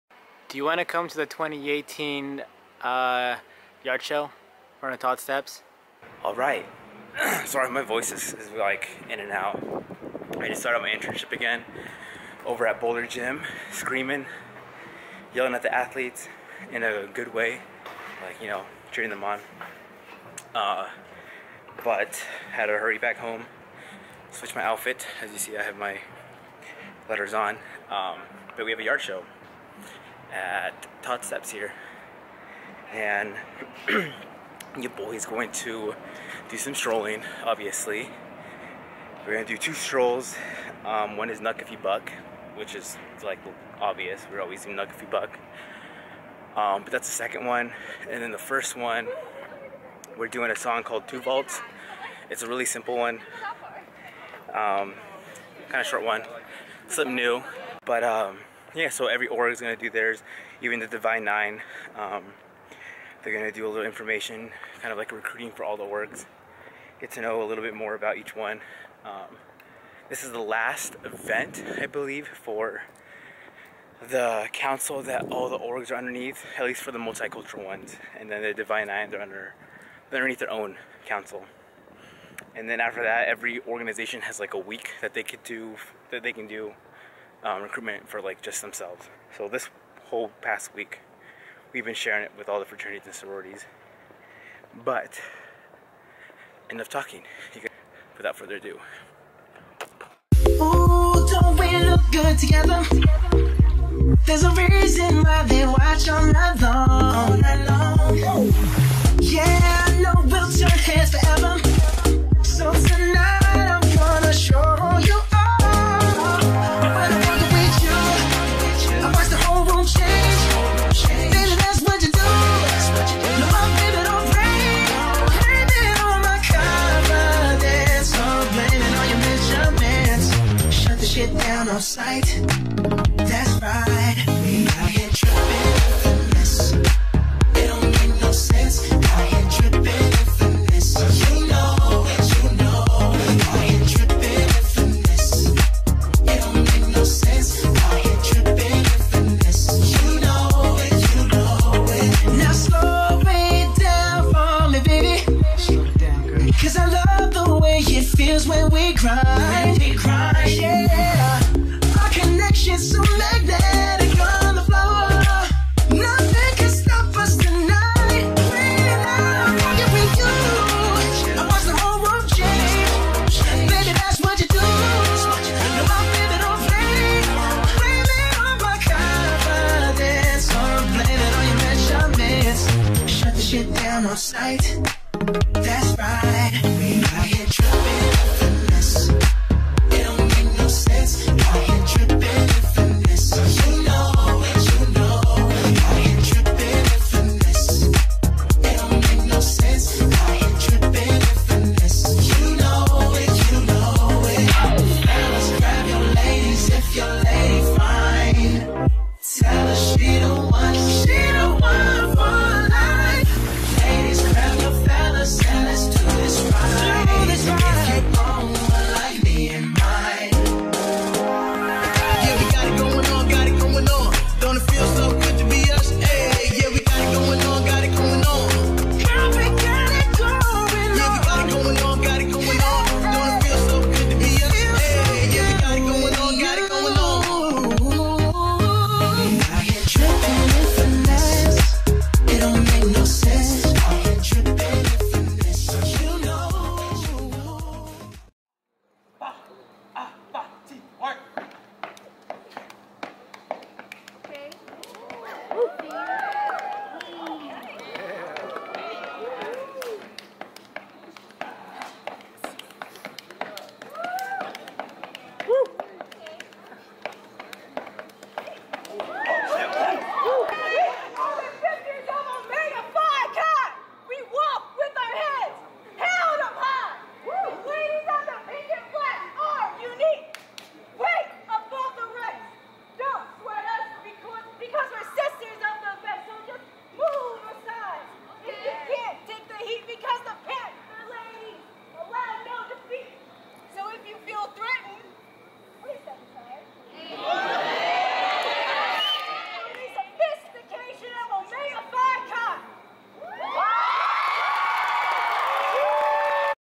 Multicultural Greek Yardshow 2018
Any negative comments to anyone in the video or to other individuals commenting will be banned without hesitation. I will do my best to remove the individuals who try to put others down in any type of form. Individuals Help Others

It's the annual Multicultural Greek Yardshow. Here is a short clip of multiple Greek Organizations that performed at this years Yardshow in front of Todd Steps. It was an amazing experience to see old faces, as well as new faces, hit the dance floor. Excited to see this community grow!

“Angep” Save 20%